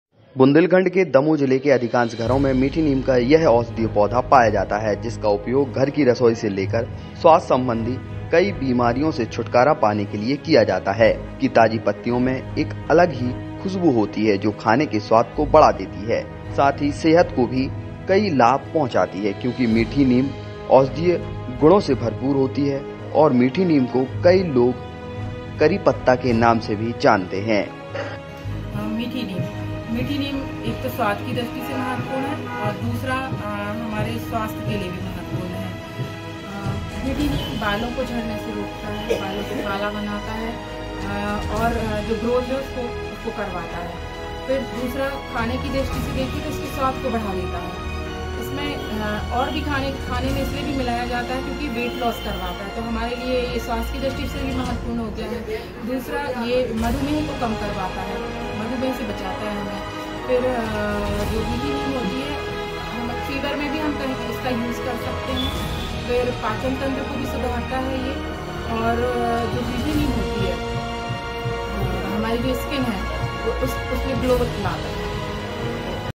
Health News: करी पत्ते के हैं अनेक फायदे, कई बीमारियों के इलाज में रामबाण
Damoh News: बुंदेलखंड के दमोह जिले के अधिकांश घरों में मीठी नीम औषधि पौधा पाया जाता है. इसका उपयोग घर की रसोई से लेकर स्वास्थ्य सम्बंधी कई बीमारियों से छुटकारा पाने के लिए किया जाता है. इसकी ताजी पत्तियों में एक अलग ही खुशबू होती है जो खाने के स्वाद को तो बढ़ाती ही है, साथ ही सेहत को भी कई लाभ पहुंचाती है
Damoh News: Sweet Neem plant is found in most of the houses in Damoh district. Its use can be obtained from many tablets from home kitchen to health expert. There is a distinct similarity in its freshness which not only enhances the taste of food but is also very beneficial for health.